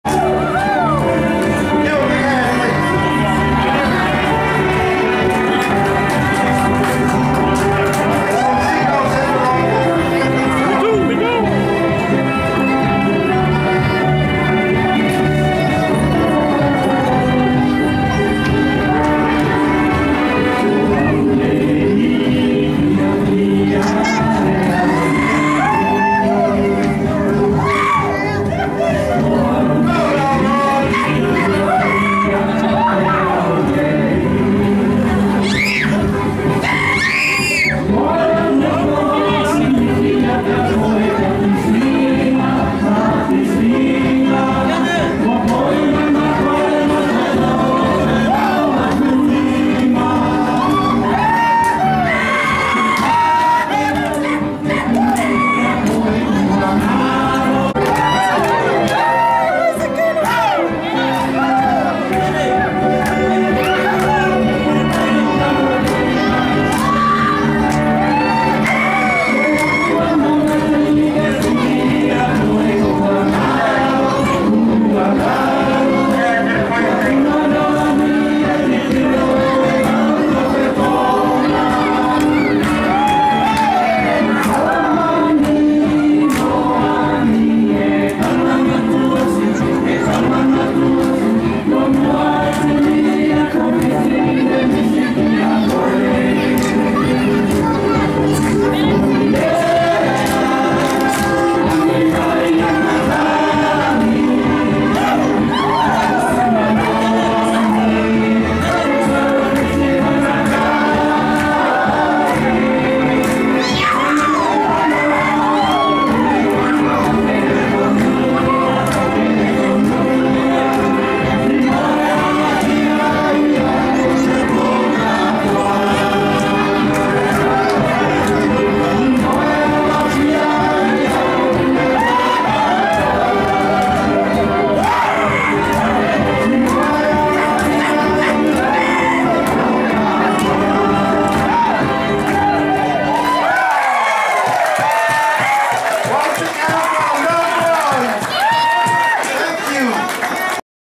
Niuean Dance Meleseine Palalagi Hilihetule Patolo 90th
Aunty Ine's 90th birthday celebration